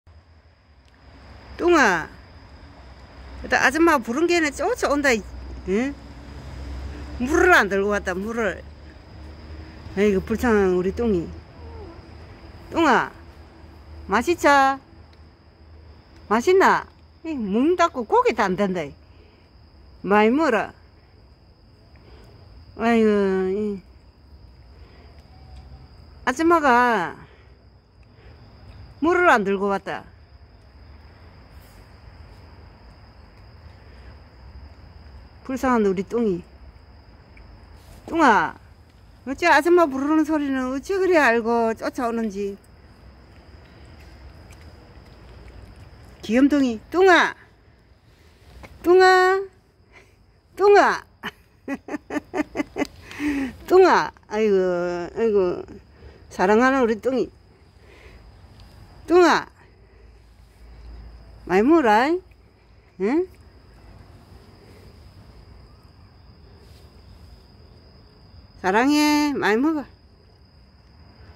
길냥이꼬리잘린 흰둥이 누가키우다가 버리고가서요 그래서마음이 안타가워요 흰둥아부르면 달려와반겨줘요 사랑스럽고 예쁘요#
길냥이중에 흰둥이 약자예요 경계심 두러움 겁이많아요 벌써흰둥이 밥챙겨준지 4년째네요 제가 집에 데리고와 키울려고 해도안되고 항상 자식처럼 손가락아픈 흰둥이입니다 지금처럼 안아프고 잘자라서면해요 흰둥이 입맛이 까달로와요 아무게나 안먹어요캔 빼로빼로 잘먹어요 사료만주면 잘안먹어요